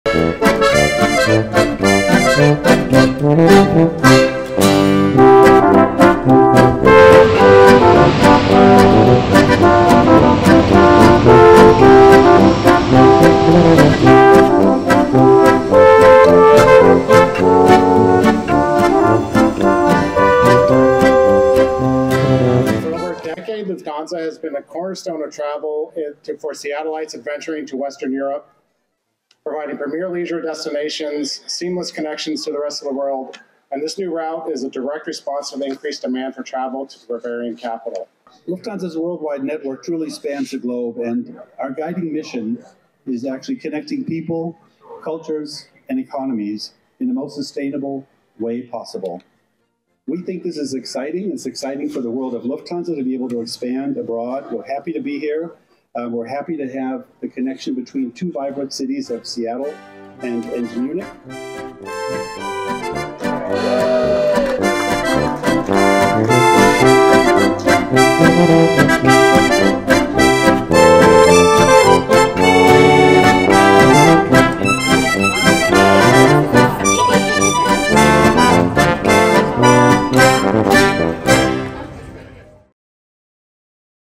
Bound for Bavaria | Lufthansa's Inaugural Flight to Munich from SEA
On May 31, 2024, Seattle-Tacoma International Airport (SEA) welcomed Lufthansa’s new nonstop flight from Munich, Germany. Kicking off new service to the Bavarian capital. This continues SEA’s streak of new international services announced and starting in 2024. Currently, a total of seven new services are expected to begin in 2024, compared to three in 2023 and four in 2022.

Lufthansa announced the nonstop route in September 2023, making it SEA’s second nonstop German destination after existing service to Frankfurt on both Lufthansa and Condor. Munich will offer even greater connections to Europe and beyond. To recognize this important step in connecting our two cities, especially the opportunity it presents for our local economy, SEA and Lufthansa held a ribbon cutting and celebratory event at the arrival gate.